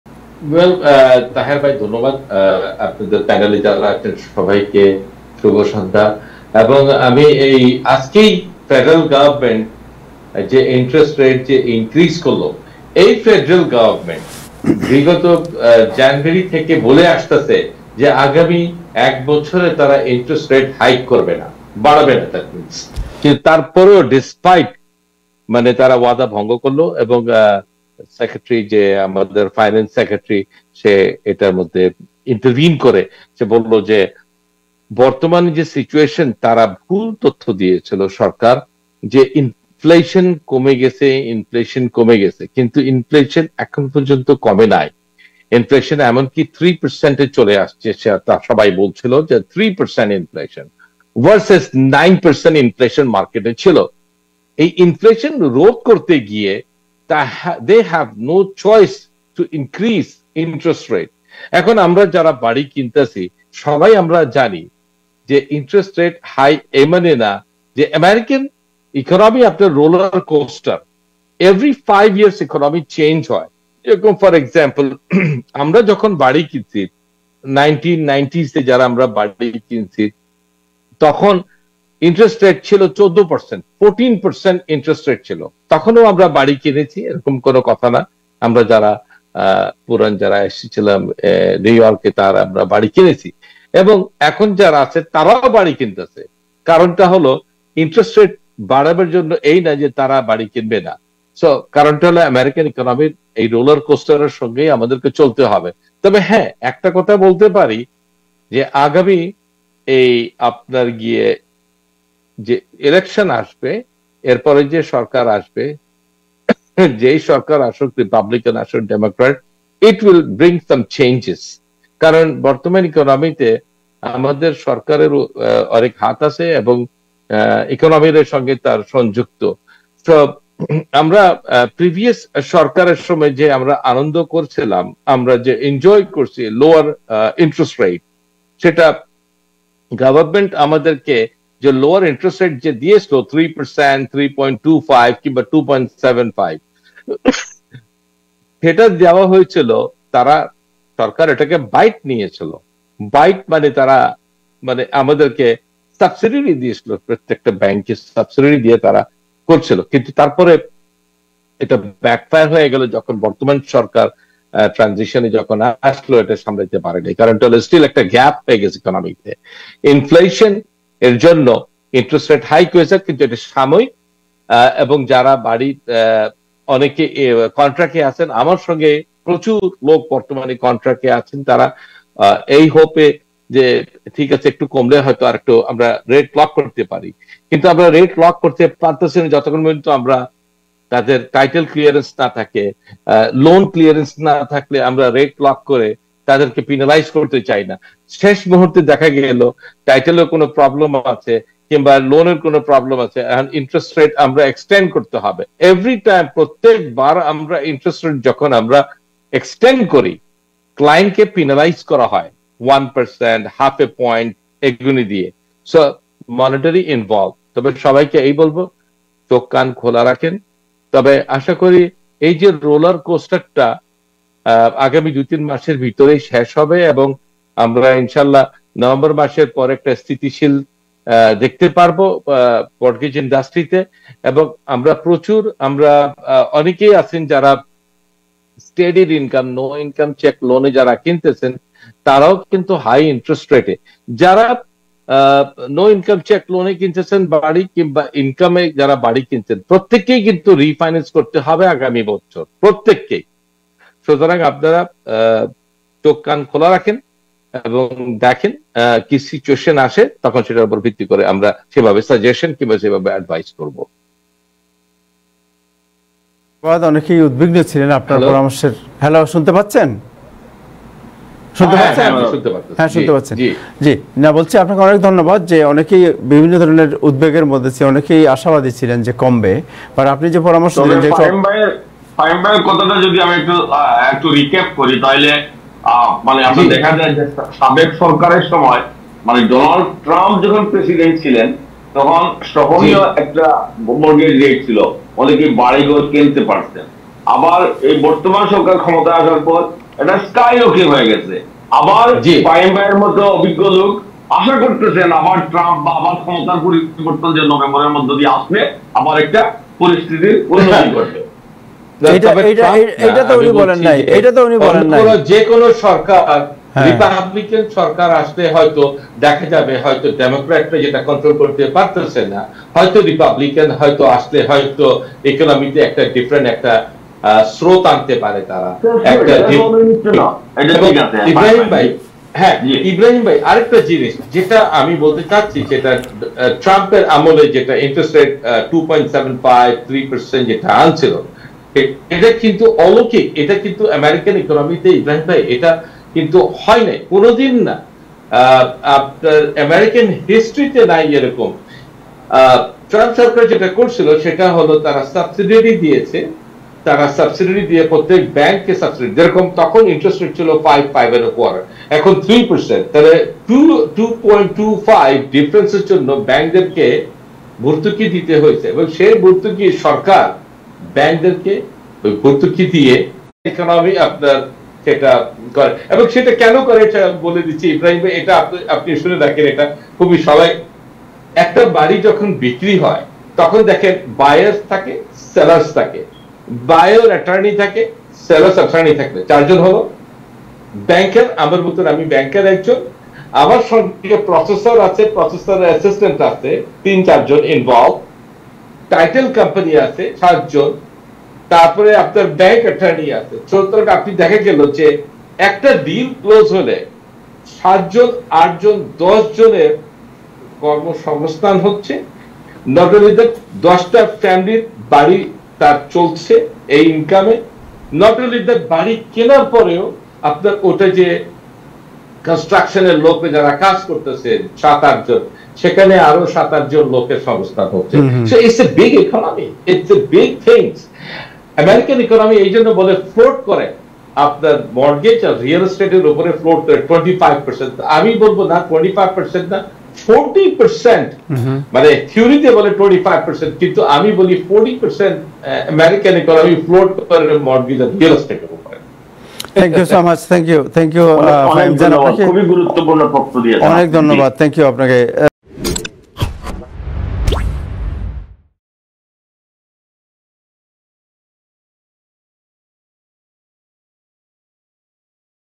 উচ্চ সুদের হার অব্যাহত রাখার সিদ্ধান্ত: বাড়ির মালিক ও নতুন ক্রেতাদের মধ্যে হতাশা ও ক্ষোভ
উচ্চ সুদের হার অব্যাহত রাখার সিদ্ধান্ত: বাড়ির মালিক ও নতুন ক্রেতাদের মধ্যে হতাশা ও ক্ষোভ | Time Television

Guest: Mohammed Fahim Jan
Host: Abu Taher